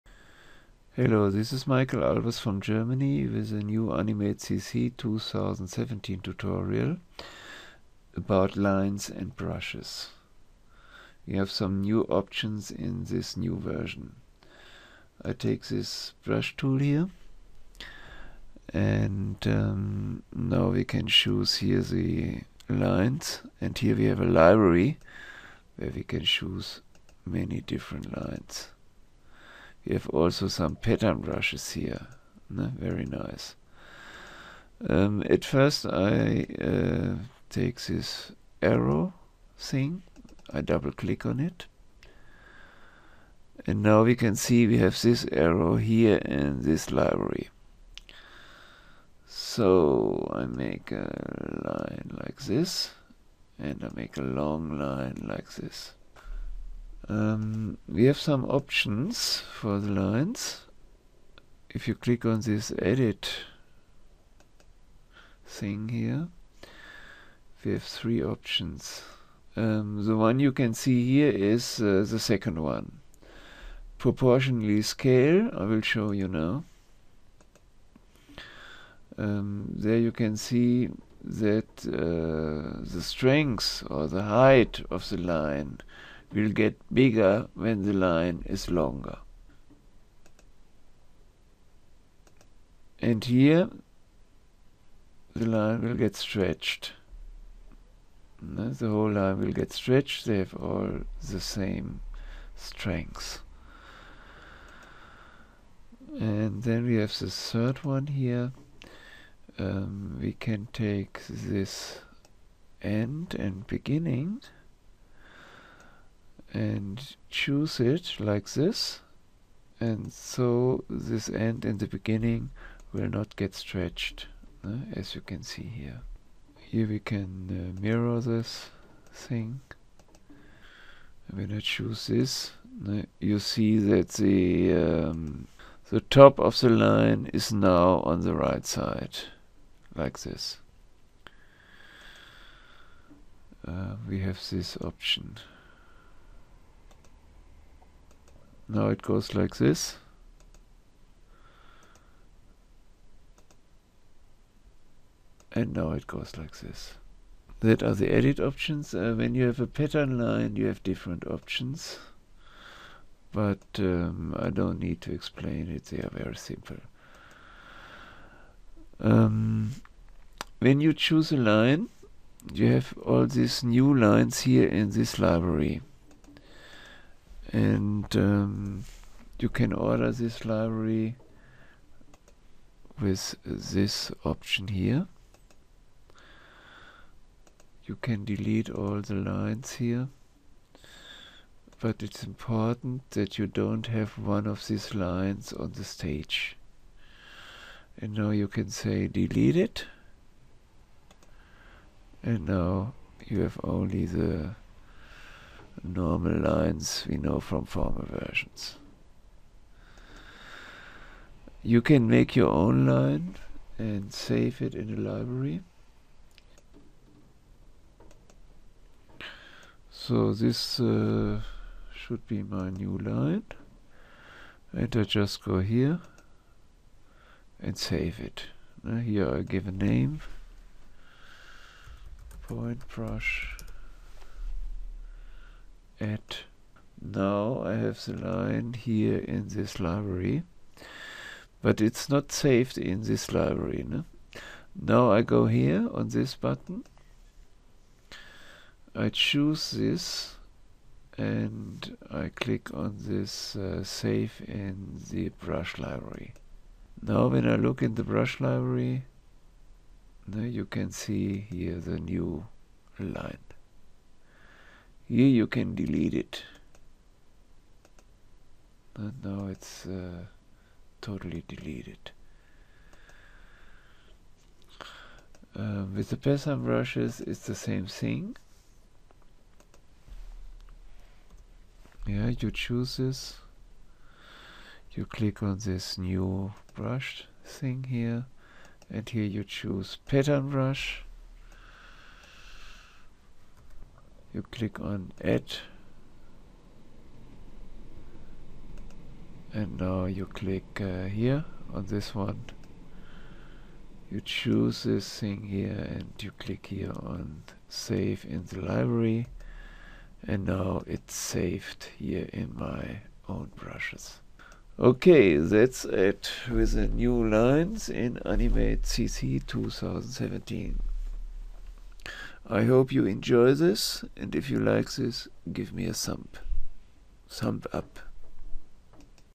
Animate CC 2017 new Brushes
In this AnimateCC tutorial I show the new brush features and how to make your own brush.